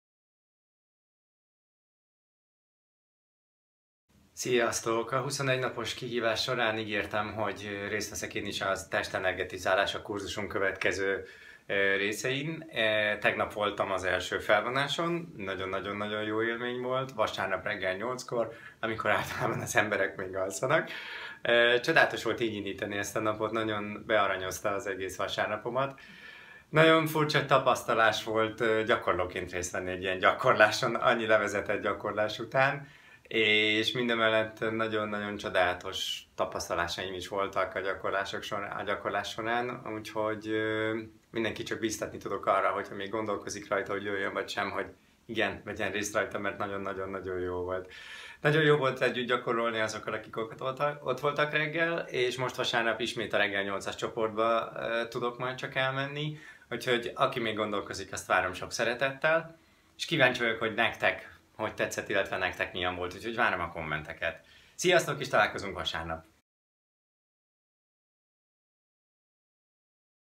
Gábor a kurzuson
A test energetizálása kurzusunk szuper!
Én már csak tudom, hiszen ott voltam gyakorlóként.
Nektek milyen volt?